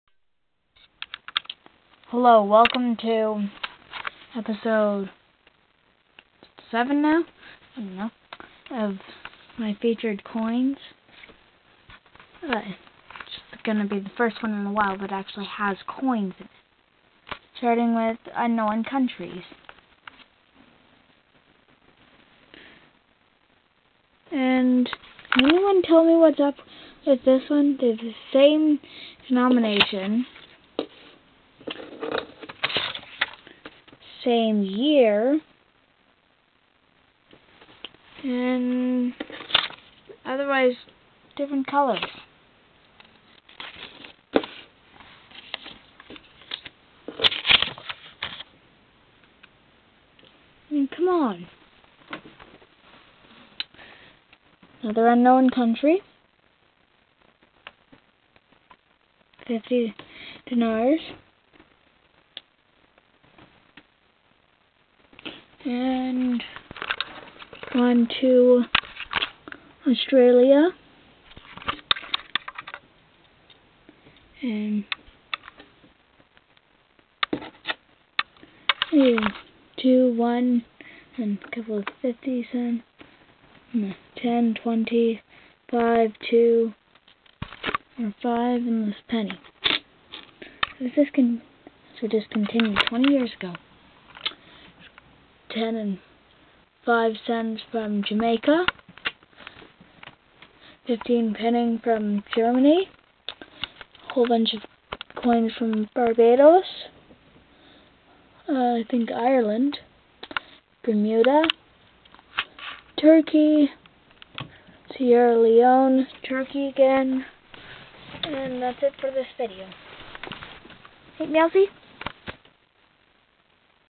My Featured Coins: Episode 7 - Multiple Countries
Coins are back! I hope that you like this video! The title was too long, so the countries that are in this video are: Barbados, Germany, Sierra Leone, Jamaica, Australia, Bahamas, and Yugoslavia.